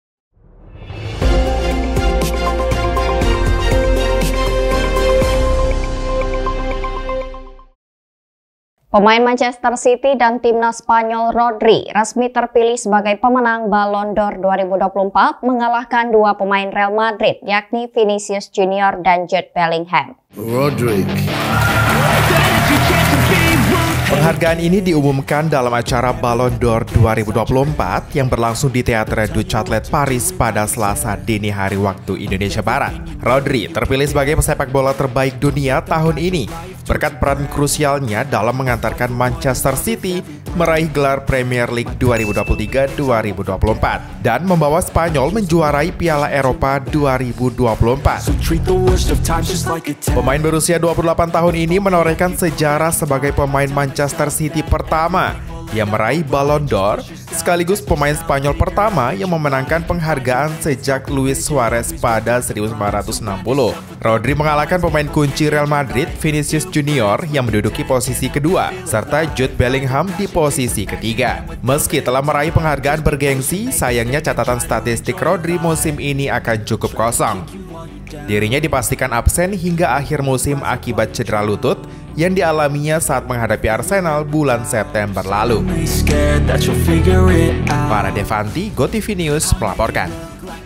Rodri Raih Ballon d’Or 2024, Langsung Cetak Rekor!
GOTVNEWS, Paris - Pemain Manchester City dan Timnas Spanyol, Rodri, resmi terpilih sebagai pemenang Ballon d’Or 2024, yang mengalahkan dua pemain Real Madrid, Vinicius Junior dan Jude Bellingham.